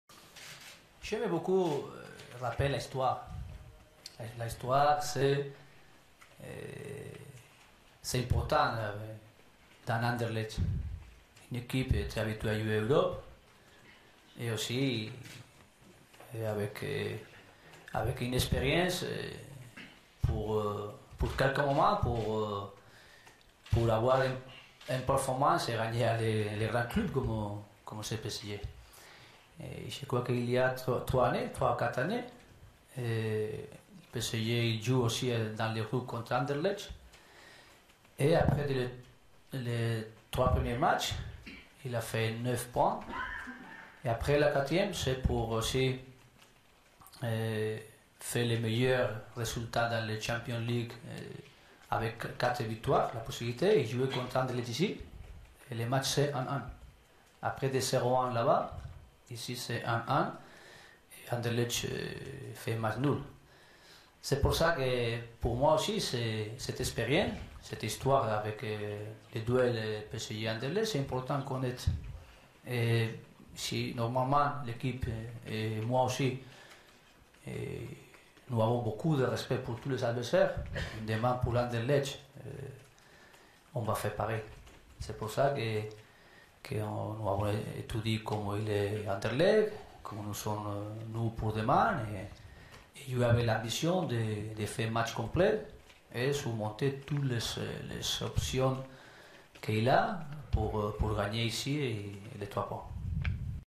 PSG : Emery «Beaucoup de respect pour Anderlecht»
FRENCH- En conférence de presse, l'entraîneur du PSG Unai Emery est revenu sur la manière dont le PSG va aborder son match contre Anderlecht.

ENGLISH- At a press conference, PSG coach Unai Emery came back on the way PSG will tackle his match against Anderlecht.

SPANISH- En una conferencia de prensa, el entrenador del PSG, Unai Emery, volvió sobre la forma en que el PSG enfrentará su partido contra el Anderlecht.